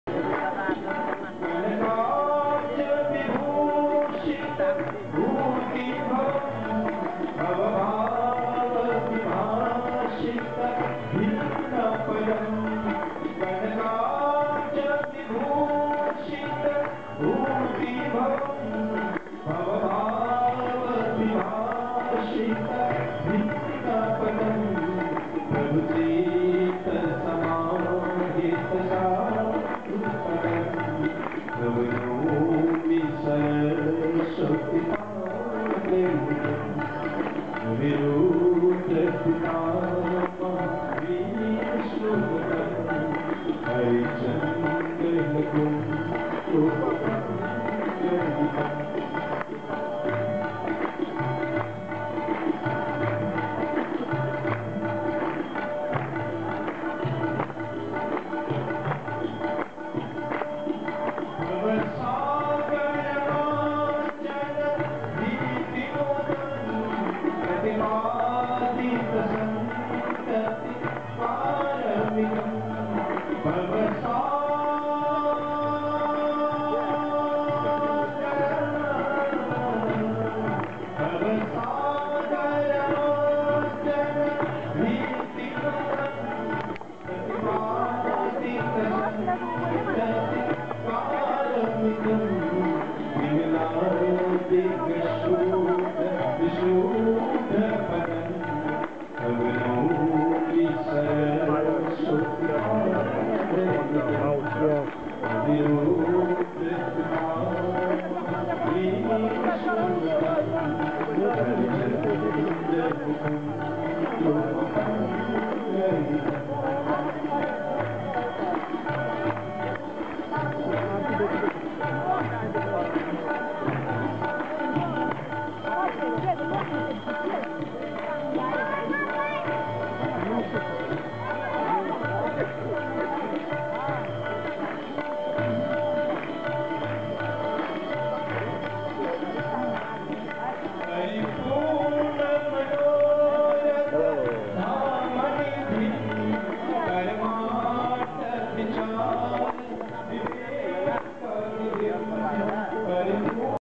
Burn Up at the Monkey temple! (shocking but beautiful)
Went down to the monkey temple on the Holy River one evening while staying at the Yak and Yeti. It was so beautiful and peaceful down there.